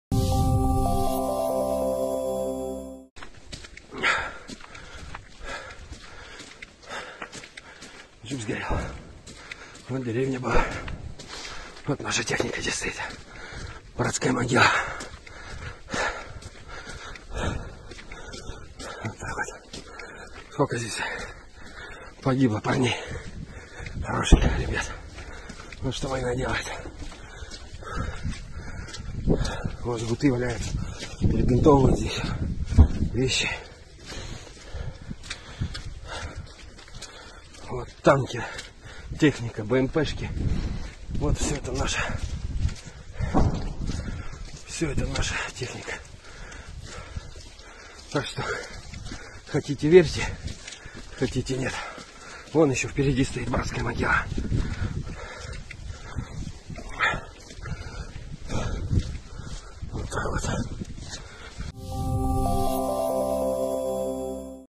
“This is cemetery, our soldier’s dead, equipment’s destroyed" -Russian soldier shows hit equipment
Soldiers of the invading Russian army have shared images of destroyed combat vehicles near Avdiivka city in eastern Donetsk region. As can be seen from the released footage, a large number of destroyed tanks and other armored combat vehicles have been left in the area. The invaders said they lost many fellow soldiers along with the military equipment. They called the place a "fraternity cemetery".